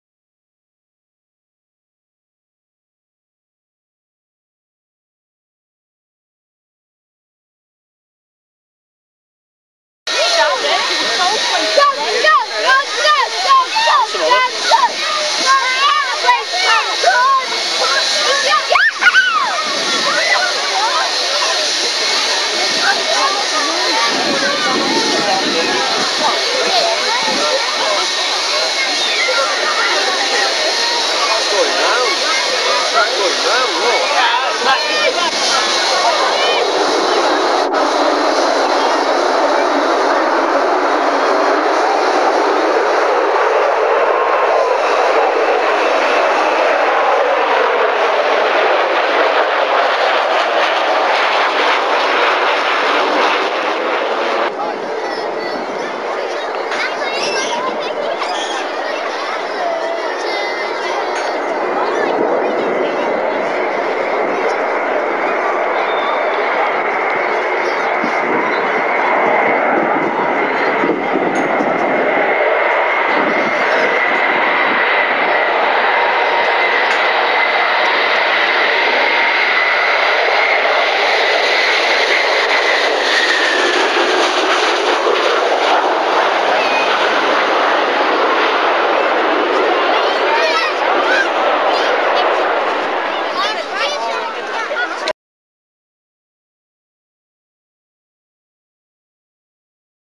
Concorde landing at Manchester Airport, UK in August, 1995.
This was filmed in August 1995 from the beer garden of the Airport Hotel pub.